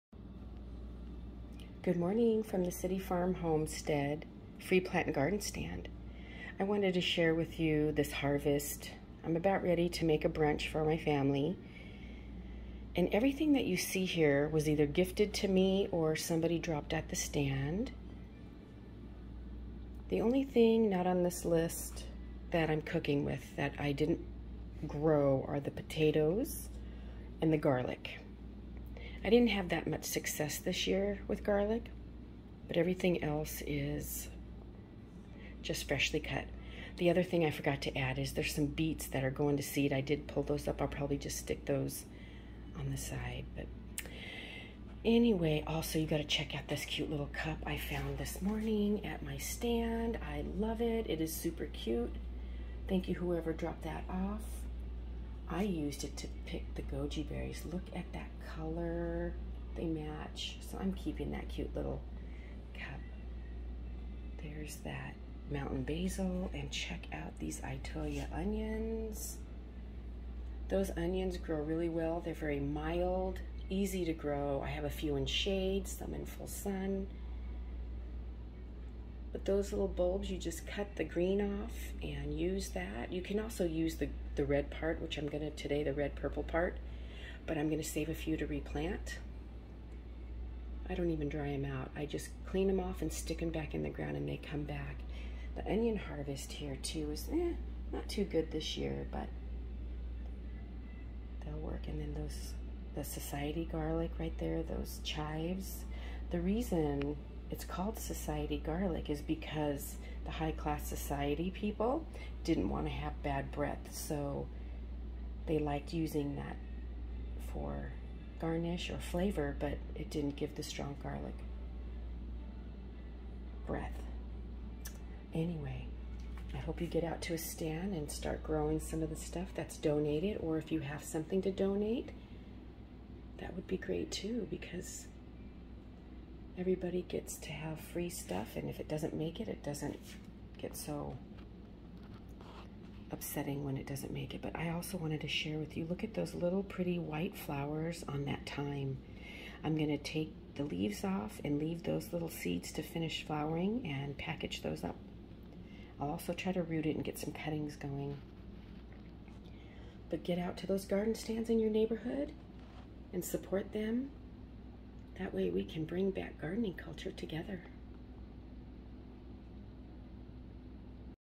Farm to table family brunch. From donations and easy to grow i'itois onions to thyme.
visit you neighborhood Free Plant N Garden Stands for fun treasures.  You never know what you'll find that another generous gardener has donated. From Chaya spinach,  goji berries,  i'itois onions, thyme and society garlic. Together we will bring back gardening culture